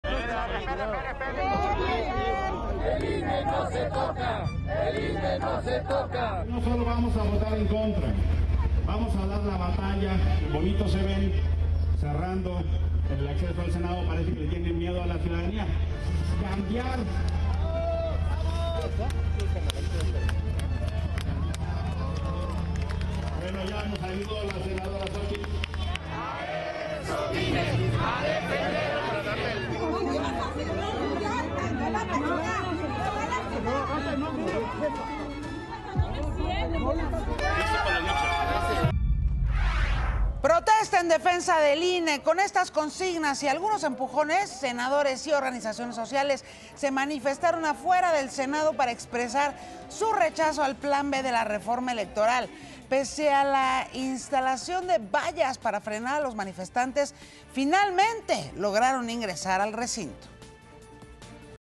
Senadores se manifiestan contra el plan B de la reforma electoral | Noticias con Yuriria Sierra
Entre consignas y algunos empujones, senadores y organizaciones sociales se manifestaron afuera del Senado para expresar su rechazo al plan B de la reformaelectoral